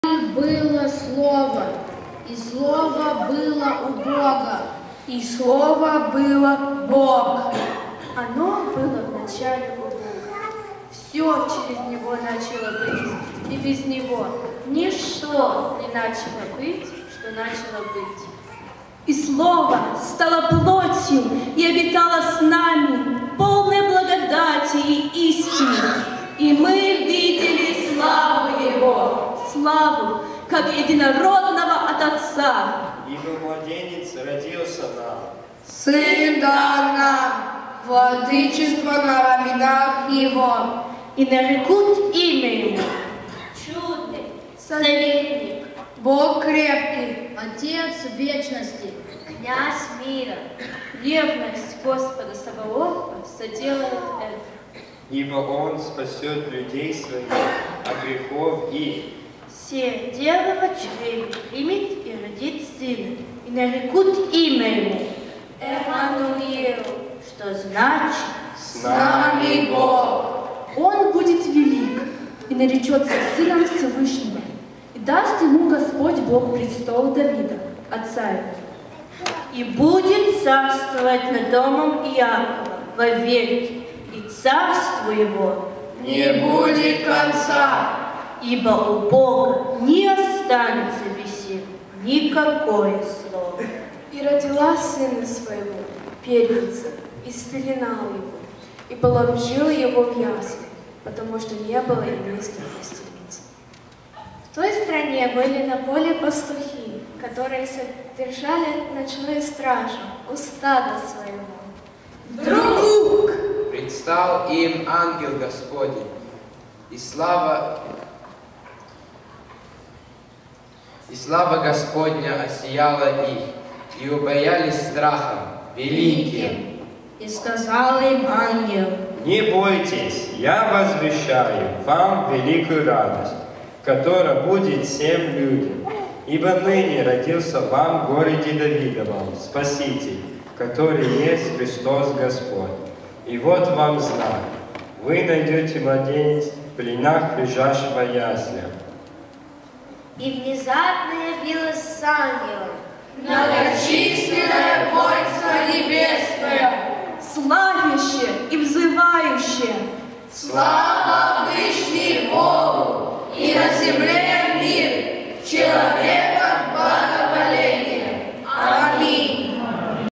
"Christmas Glory Scripture Recitation" Старшая группа. Рождественское Служение. 2019
"Christmas Glory Scripture Recitation" Старшая группа.
Рождественское Служение
San Diego Slavic Evangelical Church 12/25/2019